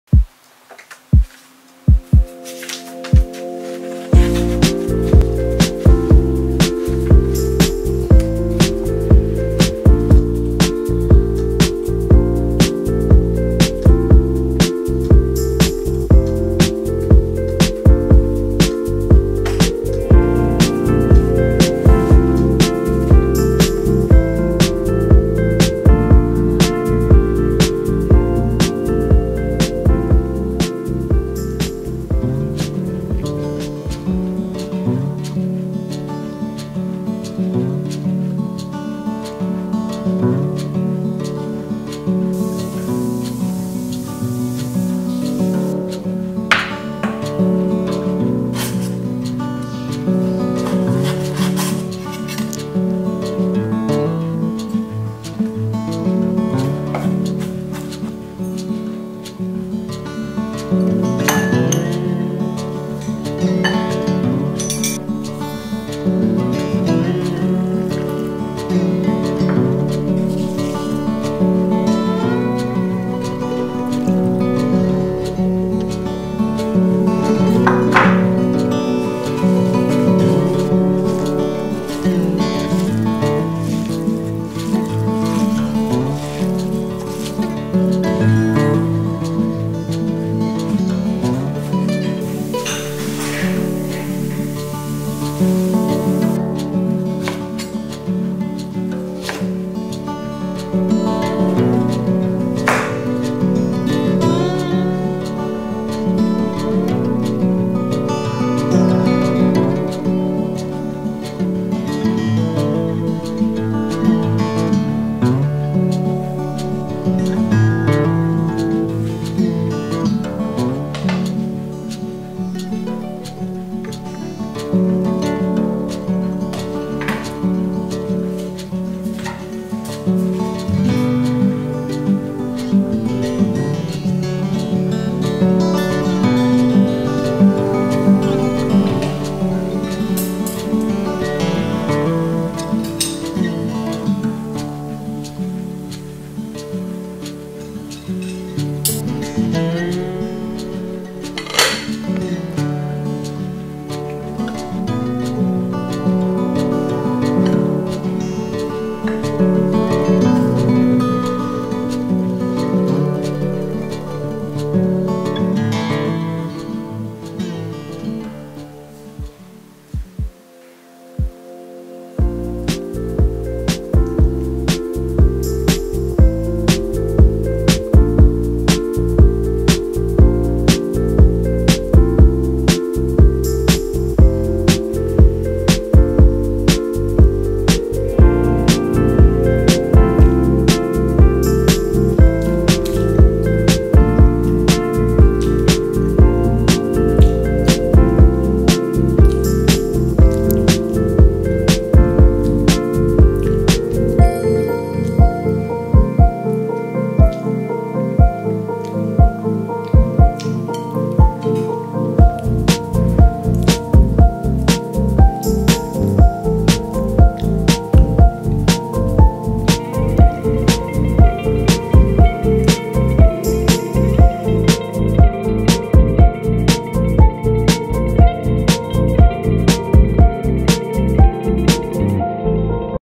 [南瓜粉蒸肉·冷食社] 最正宗的川味家常做法，甜咸交錯，肥而不膩，和記憶中的味道一模一樣
[南瓜粉蒸肉·冷食社] 小時候媽媽做的南瓜粉蒸肉總是桌上最暢銷的菜品，沒想到做法也並不複雜。
今天就教把獨家教程奉上啦！這樣做出來的南瓜粉蒸肉肥而不膩，再加上南瓜的清甜，分分鐘光碟行動！你也可以把五花肉換成牛羊等各種肉，歡迎嘗試，也歡迎來評論區與我交流~

想看到更多最新的美食教程嗎？關注\"冷食社\", 意想不到的小驚喜❤